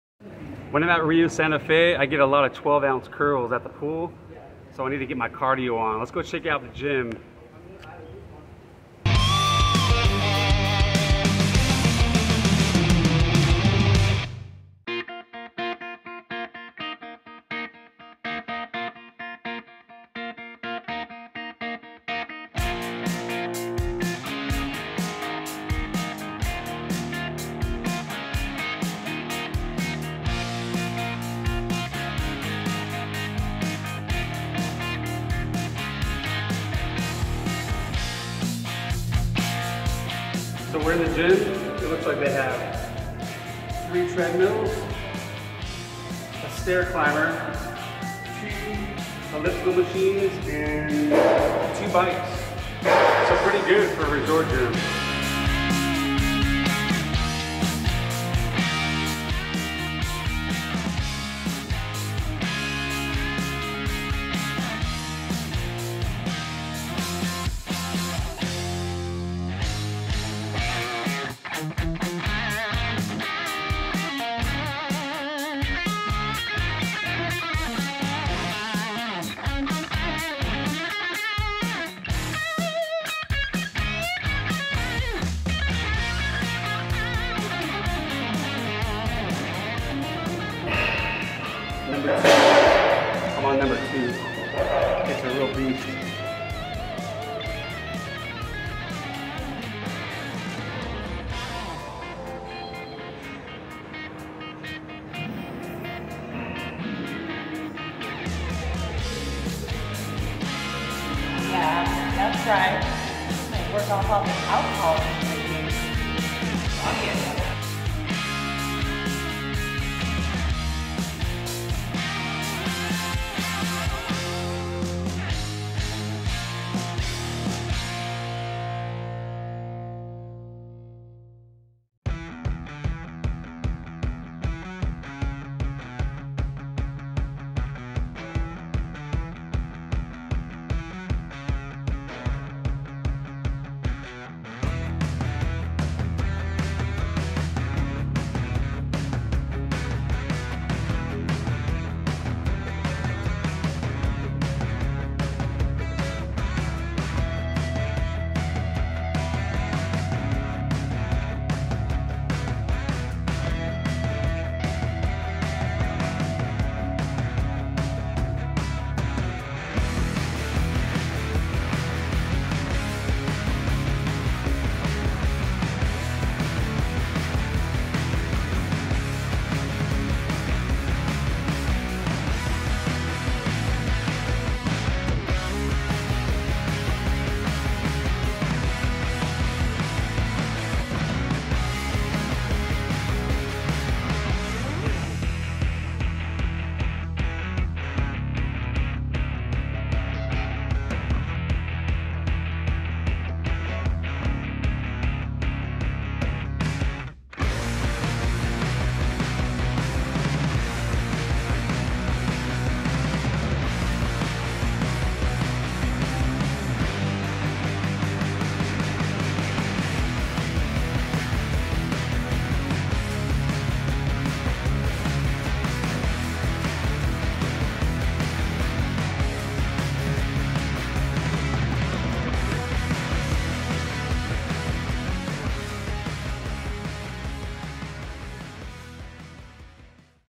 Riu Santa Fe Gym
Hey Rockers! Today we tour the Riu Santa Fe Gym and Spa in Cabo San Lucas Mexico. The all inclusive resort is the perfect vacation spot for a family or a spring break adventure.